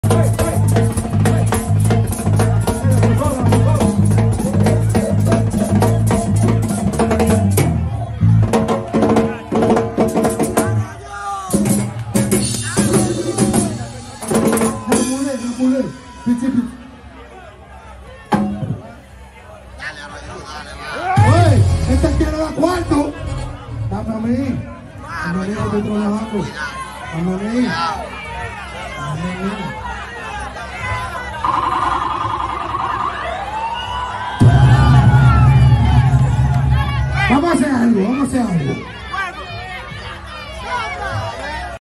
EL CATA HACE SHOW EN TARINA PATRONALES DE GUAYMATE CON AGUSTIN DIGITAL
SHOW EN TARINA CON EL CATA  PATRONALES DE GUAYMATE CON AGUSTIN DIGITAL